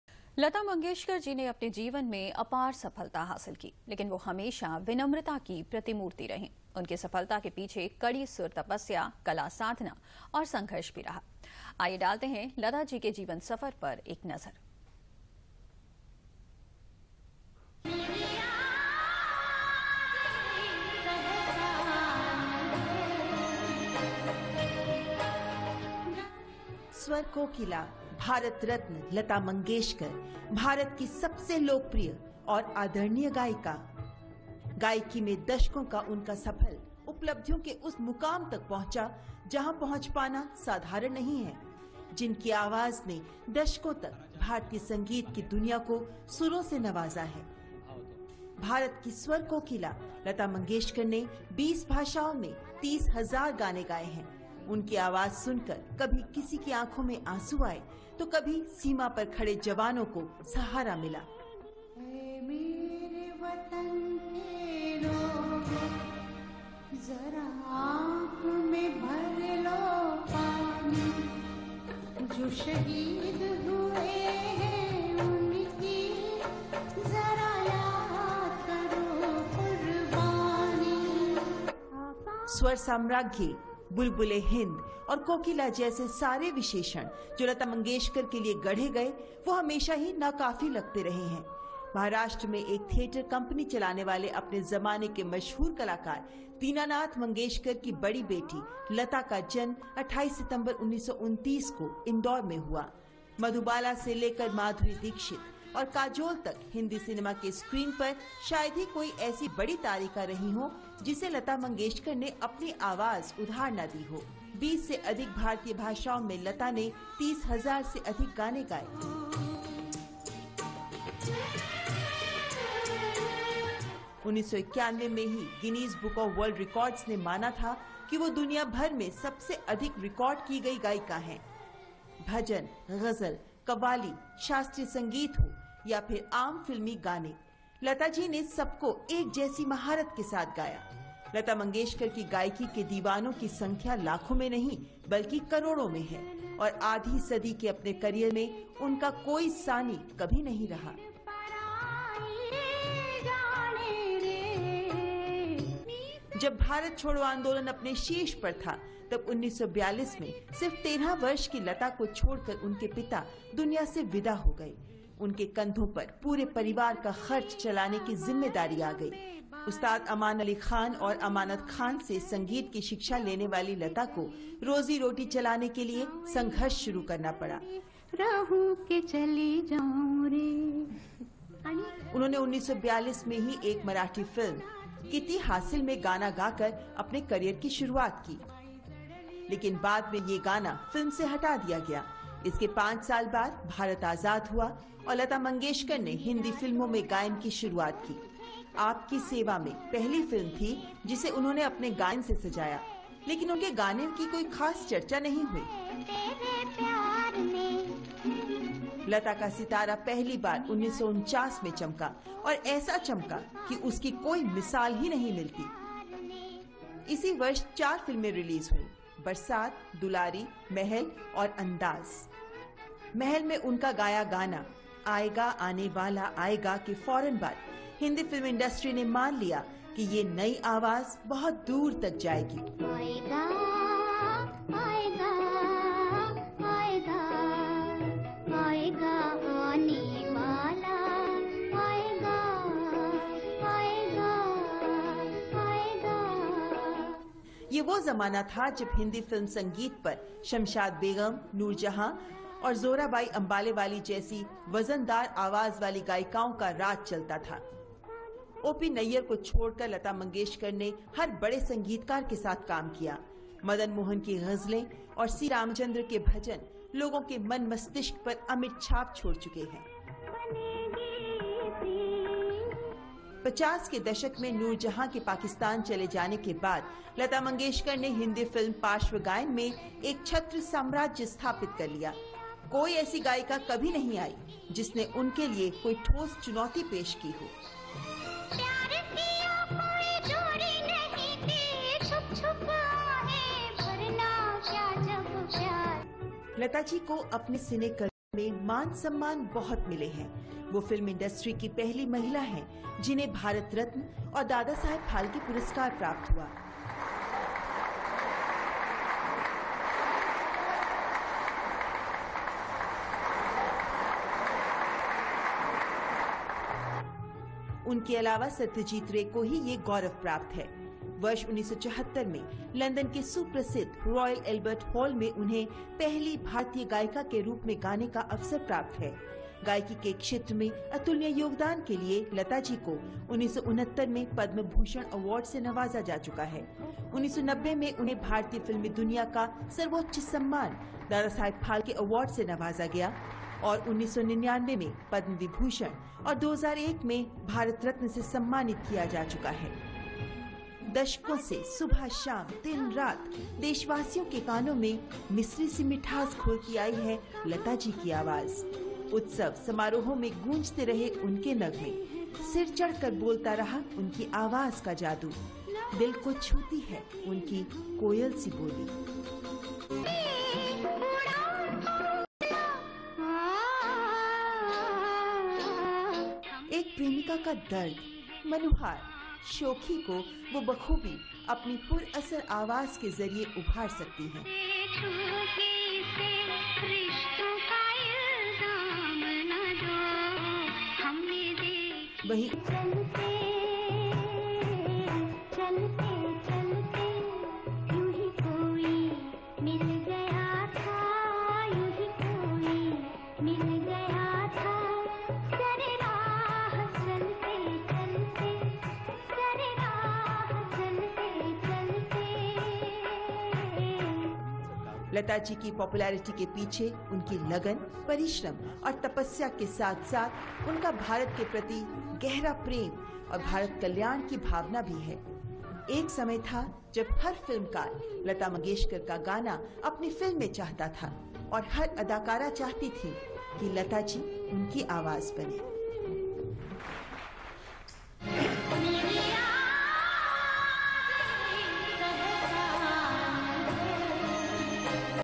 LATA MANGESHKAR JI ने अपने जीवन में अपार सफलता हासिल की
लता मंगेशकर जी ने अपने जीवन में अपार सफलता हासिल की। लेकिन वो हमेशा विनम्रता की प्रतिमूर्ति रहीं। उनकी सफलता के पीछे कड़ी सुर तपस्या, कला साधना और संघर्ष भी रहा। आइये डालते हैं लता जी के जीवन सफर पर एक नजर।